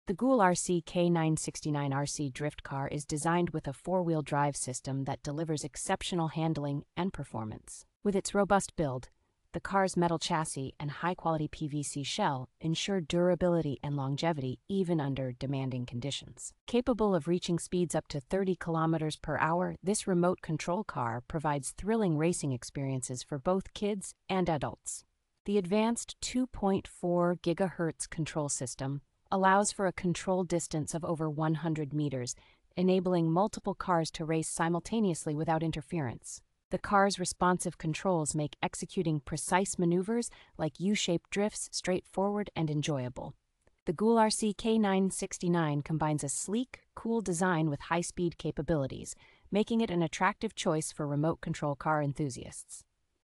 REVIEW (2025): GoolRC K969 RC Drift Car. ESSENTIAL details.
The GoolRC K969 RC Drift Car is designed with a four-wheel drive system that delivers exceptional handling and performance.

With its robust build, the car's metal chassis and high-quality PVC shell ensure durability and longevity even under demanding conditions.

Capable of reaching speeds up to thirty kilometers per hour, this remote control car provides thrilling racing experiences for both kids and adults.

The advanced two point four gigahertz control system allows for a control distance of over one hundred meters, enabling multiple cars to race simultaneously without interference.

The car's responsive controls make executing precise maneuvers like U-shaped drifts straightforward and enjoyable.

The GoolRC K969 combines a sleek, cool design with high-speed capabilities, making it an attractive choice for remote control car enthusiasts.